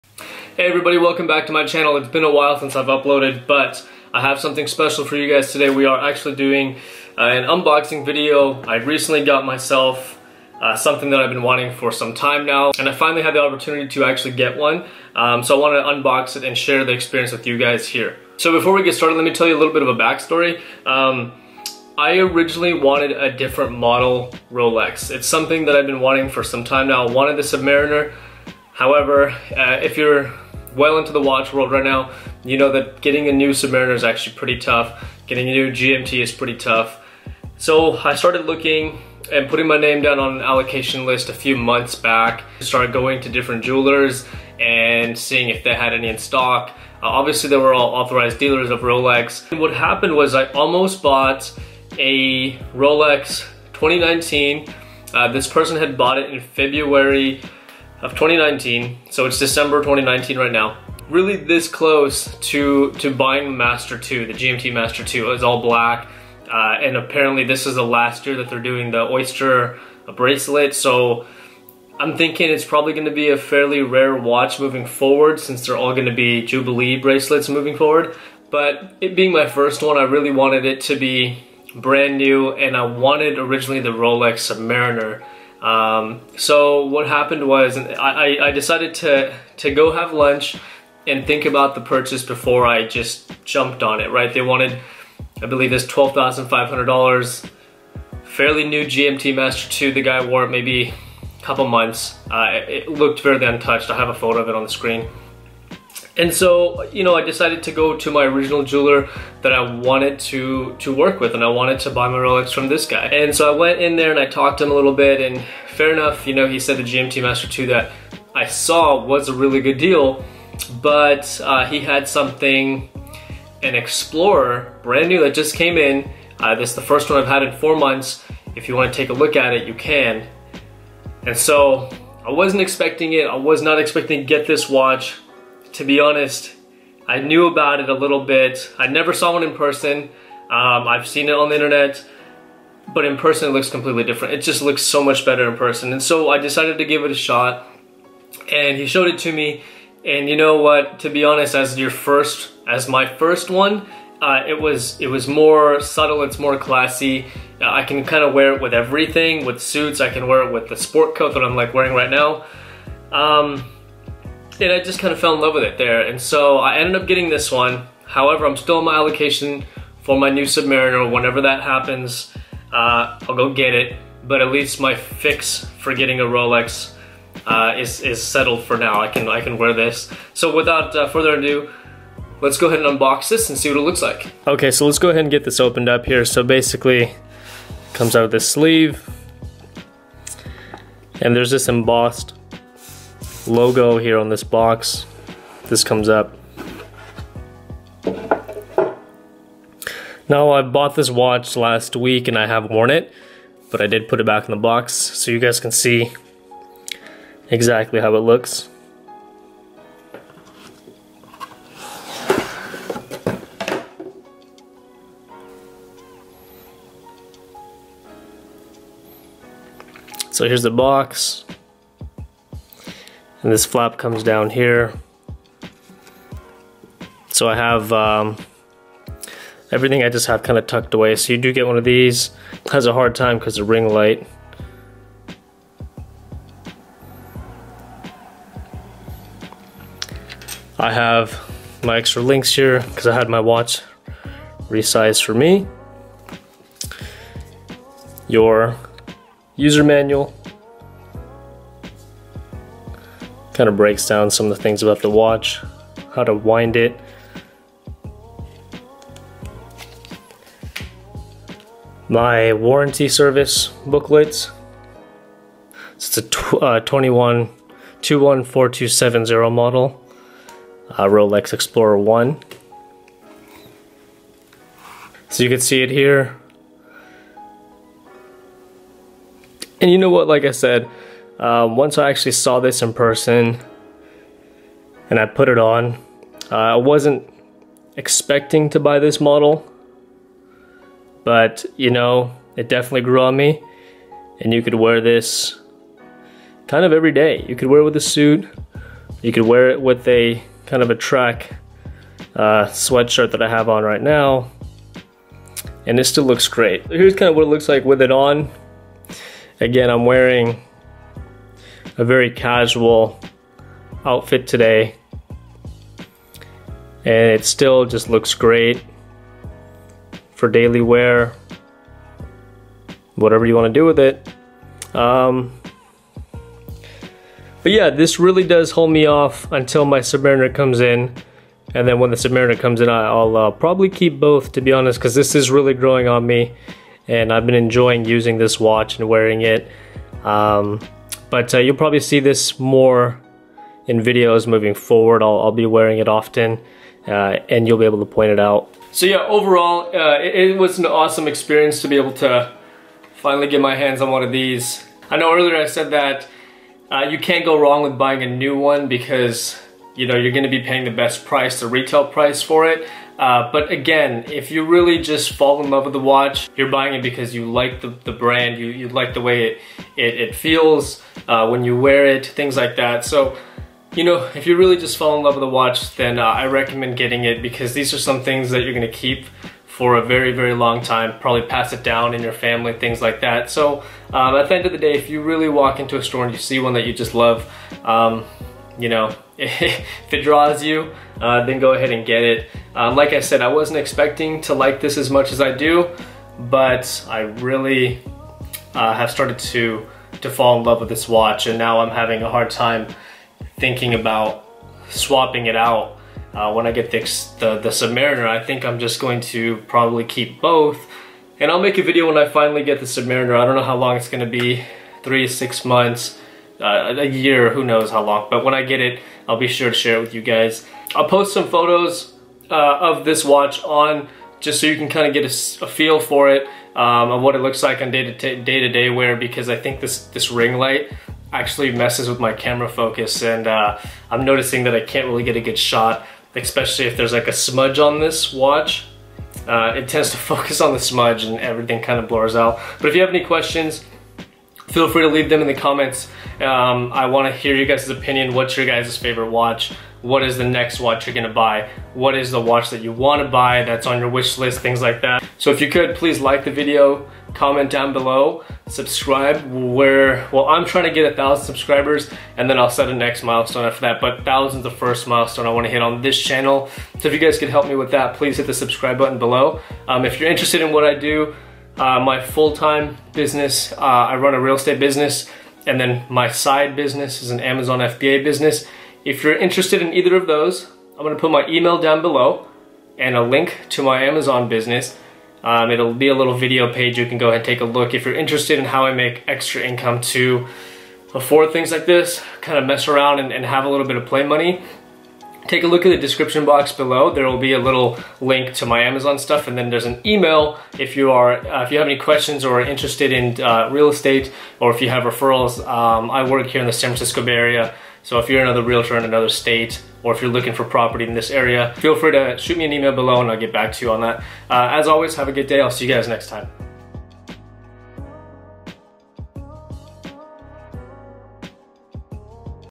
Buying My First Rolex! Unboxing & Review - Rolex Explorer 214270
After I spend more time with this watch, i'll make a follow up video of my experiences with it.

★ Have questions regarding Real Estate or Referrals?